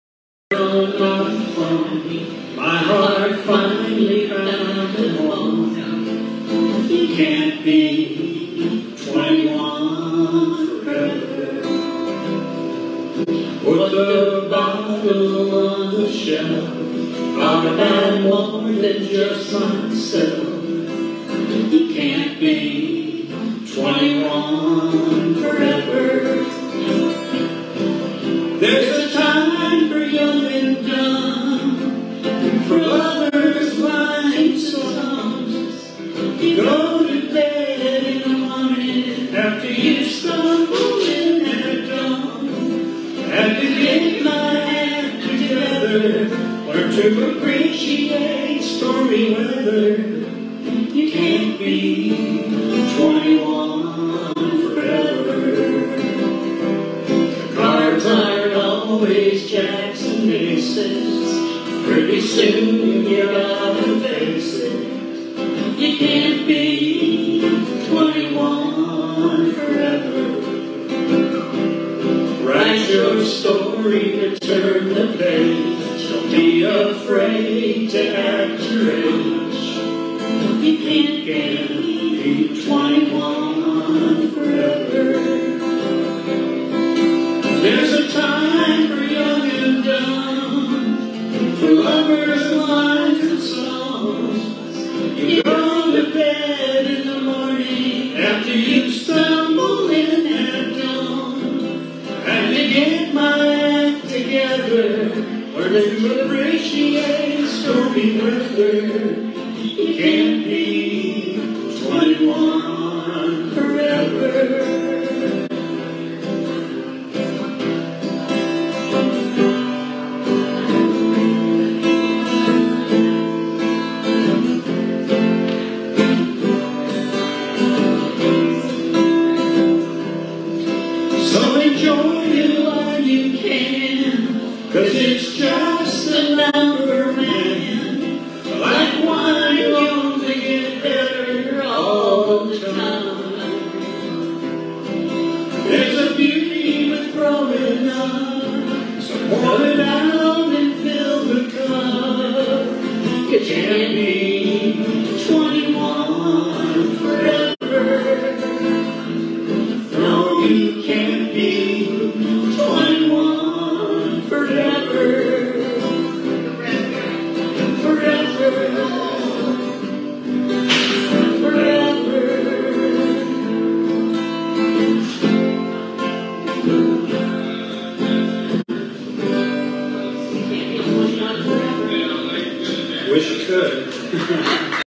21 Forever 8 12 23
Performances with Students and Families at South Bend Chocolate Cafe